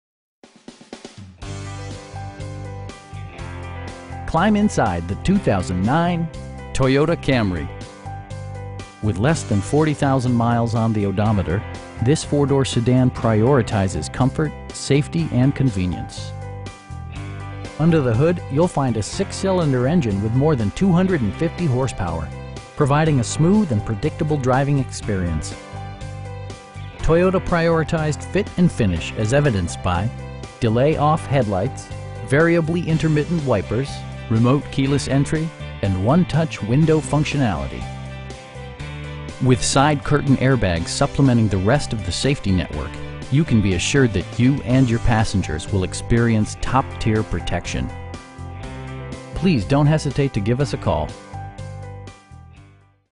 2009 Toyota Camry in Aurora, CO 80012
Stevinson Toyota Scion East
444 S Havana St.

Climb inside the 2009 Toyota Camry. With less than 40,000 miles on the odometer, this 4 door sedan prioritizes comfort, safety and convenience. Under the hood you'll find a 6 cylinder engine with more than 250 horsepower, providing a smooth and predictable driving experience. Toyota prioritized fit and finish as evidenced by: delay-off headlights, variably intermittent wipers, tilt steering wheel, power door mirrors, remote keyless entry, an overhead console, and 1-touch window functionality. With side curtain airbags supplementing the rest of the safety network, you can be assured that you and your passengers will experience top-tier protection. We know that you have high expectations, and we enjoy the challenge of meeting and exceeding them! Please don't hesitate to give us a call.